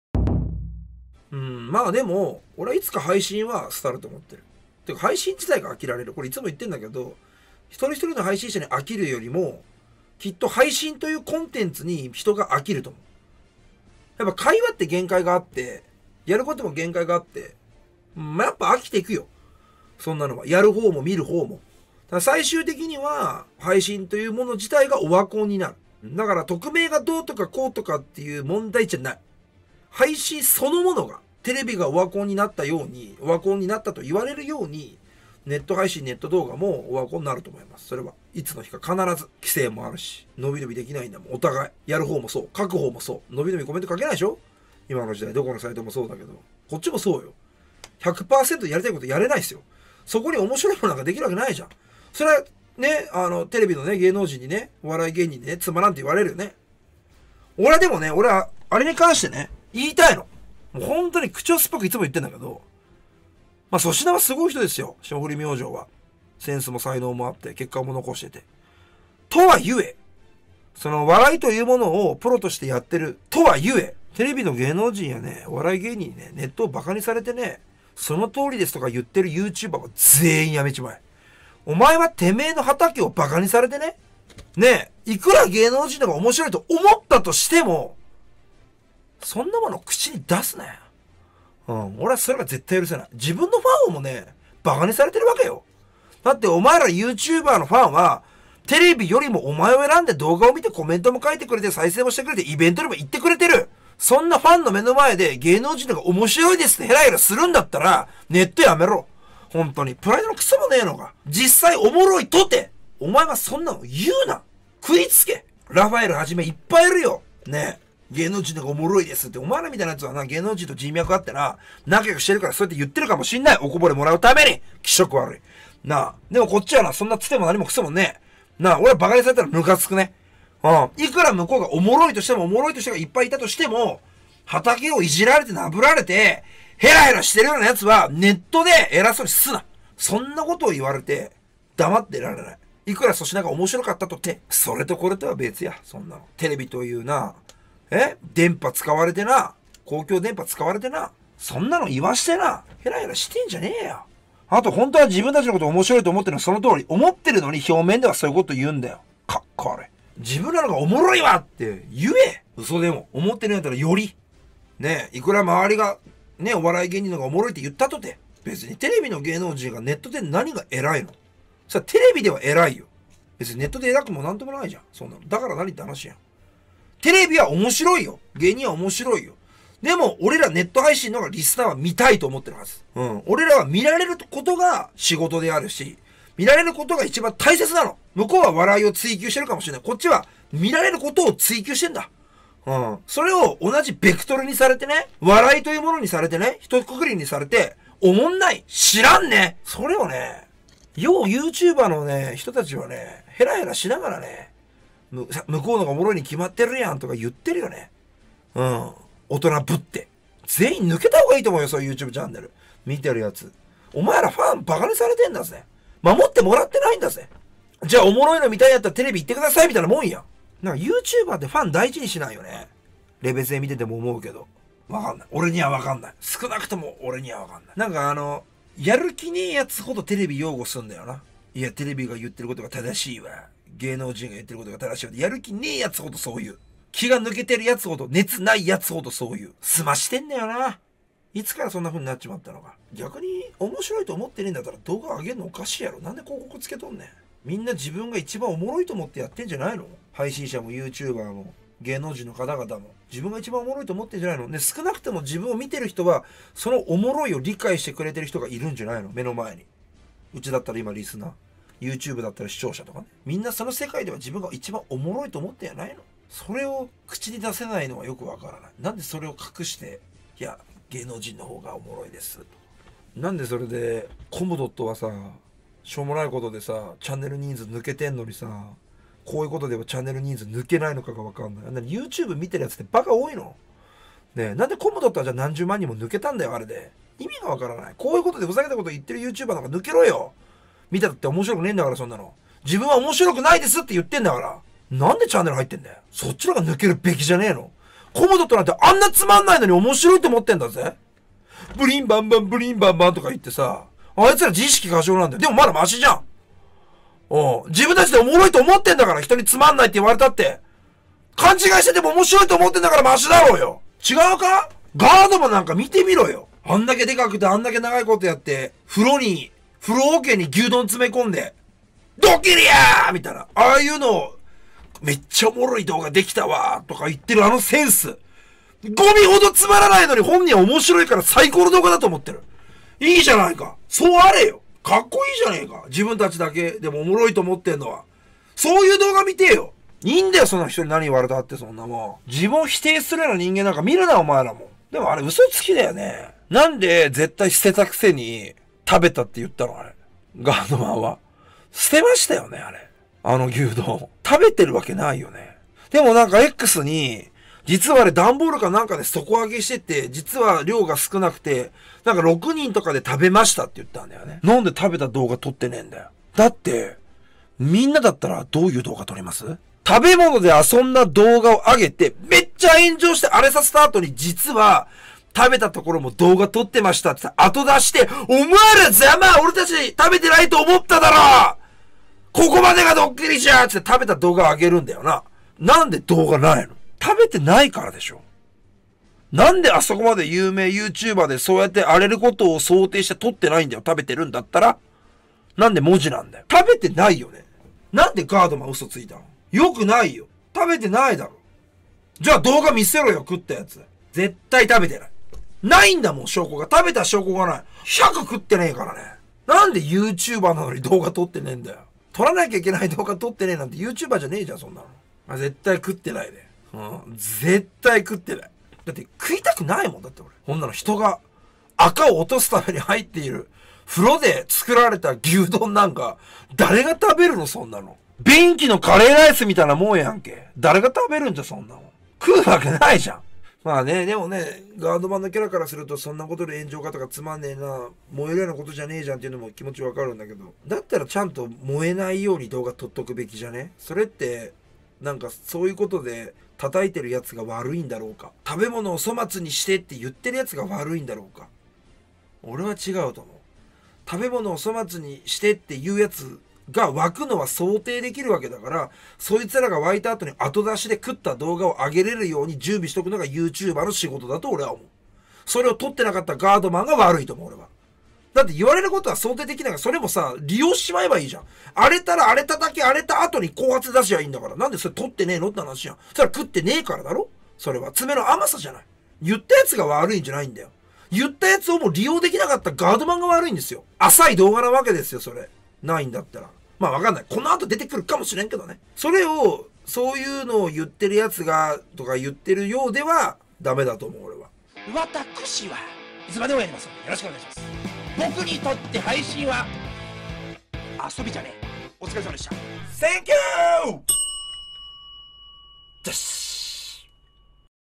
芸人に「おもんない」と言われて認めるYouTuberに言いたいこと/牛丼を浴槽に詰める動画で大炎上したがーどまんについて
放送日　2024年04月29日 定時配信　今日のニュース